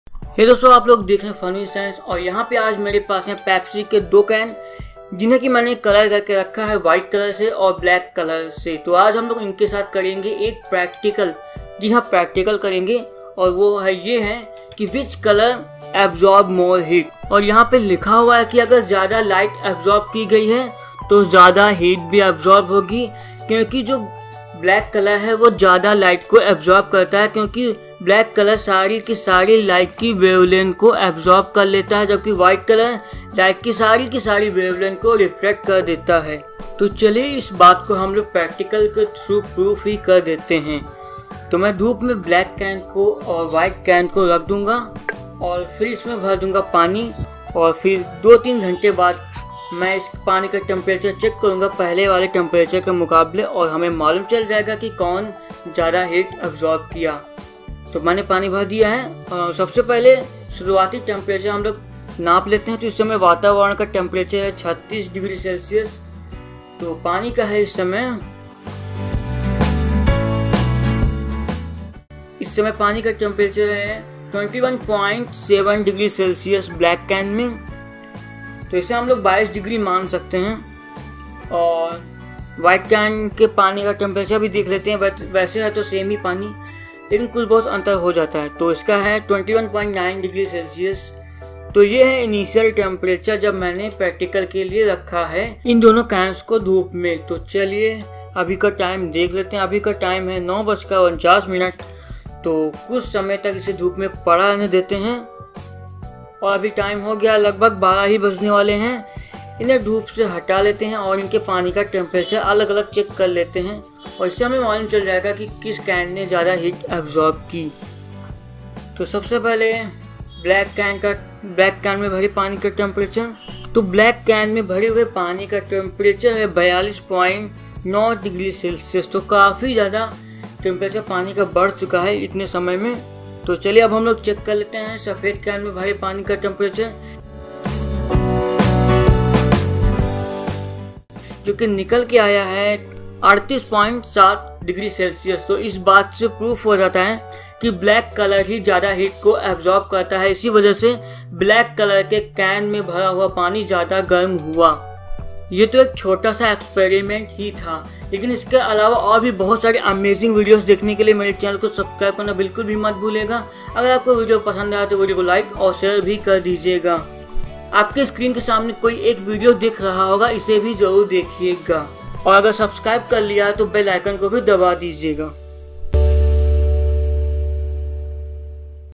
क्या आपको काले और सफ़ेद कैन का ये गुण पता है - Which Can Absorb More Heat
This is a simple experiment or simple practical video . If you are searching a video for your school college science projects then this video can give you a idea for your school science projects.
In this video am going to proof which color absorb more heat via a simple easy to do piratical.